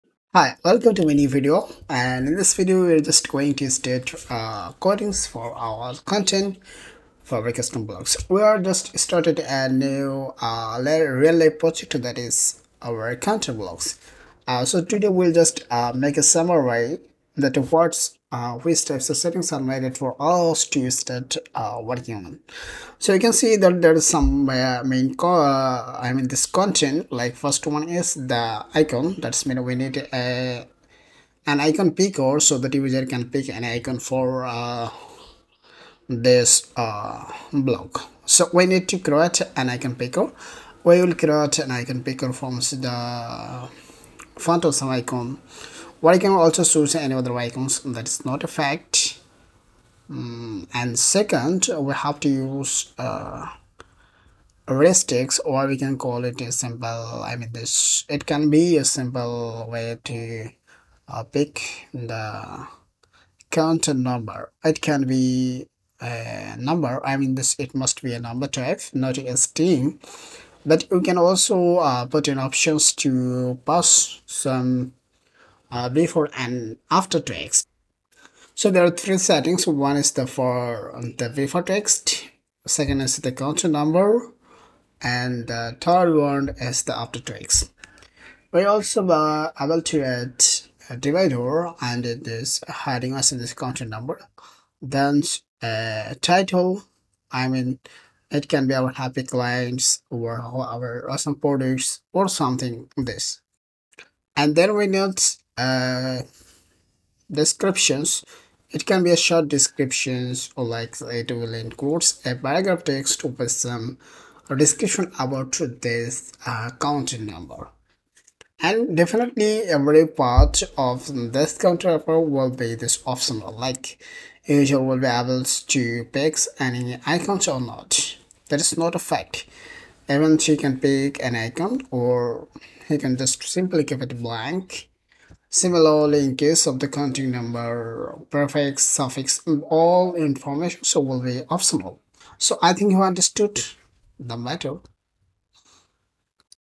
Develop Custom Gutenberg Block - Counter Block-02 | Part-31
Learn how to develop a complete custom Gutenberg block called Counter Block.

❓ NEED to redesign your website with Gutenberg Editor ???
❓ or Custom Block Development ???

Hire Me on Fiverr or UpWork:-